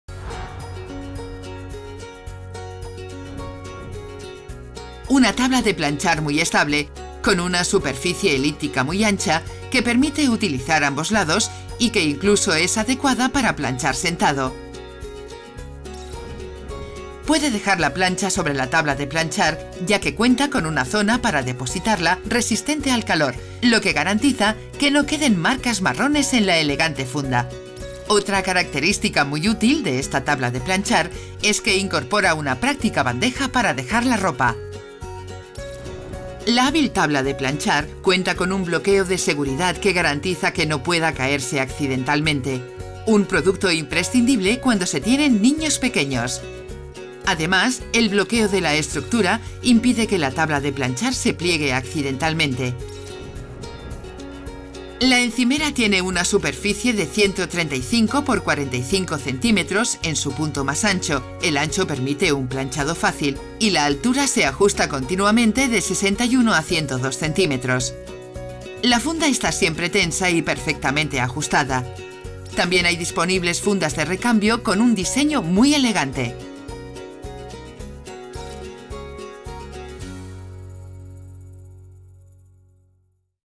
Mesa de Planchar D con Area Resistente Al Calor y Bandeja Para Ropa | Brabantia
Una tabla de planchar muy estable, con una superficie elíptica muy ancha, que permite utilizar ambos lados  y que incluso es adecuada para planchar sentado.

Puede dejar  la plancha sobre la tabla de planchar, ya que cuenta con una zona para depositarla resistente al calor, lo que garantiza que no queden marcas marrones en la elegante funda. Otra característica muy útil de esta tabla de planchar  es que incorpora una práctica bandeja para dejar la ropa.

La hábil tabla de planchar cuenta con un bloqueo de seguridad que garantiza que no pueda caerse accidentalmente; un producto imprescindible cuando se tienen niños pequeños. Además, el bloqueo de la estructura impide que la tabla de planchar se pliegue accidentalmente.

La encimera tiene una superficie de 135x45 cm en su punto más ancho —el ancho permite un planchado fácil— y la altura se ajusta continuamente de 61 a 102 cm. La funda está siempre tensa y perfectamente ajustada; también hay disponibles fundas de recambio con un diseño muy elegante.